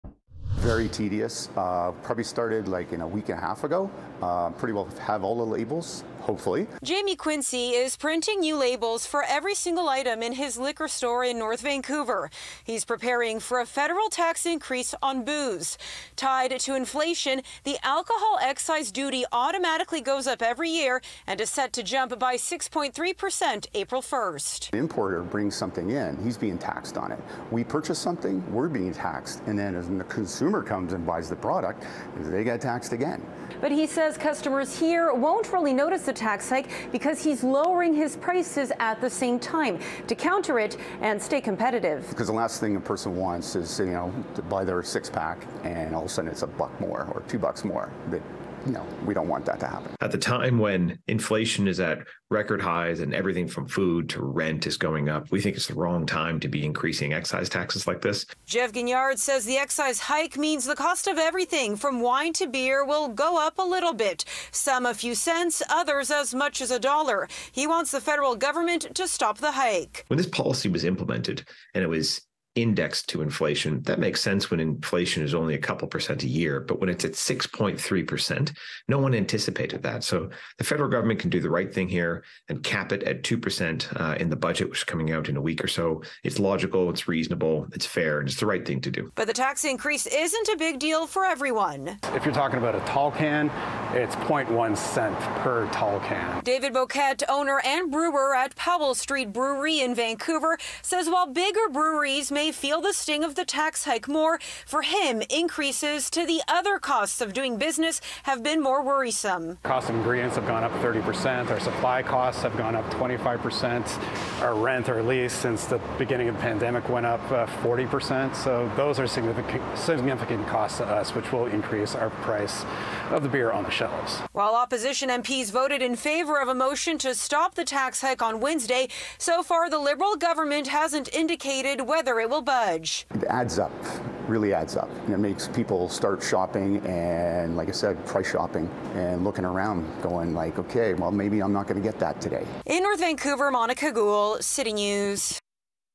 No joke: Federal alcohol tax going up April 1
A federal tax on alcohol is going up on April 1, and that’s not a joke. Monika Gul finds out what that means for local businesses, and what it could mean for you.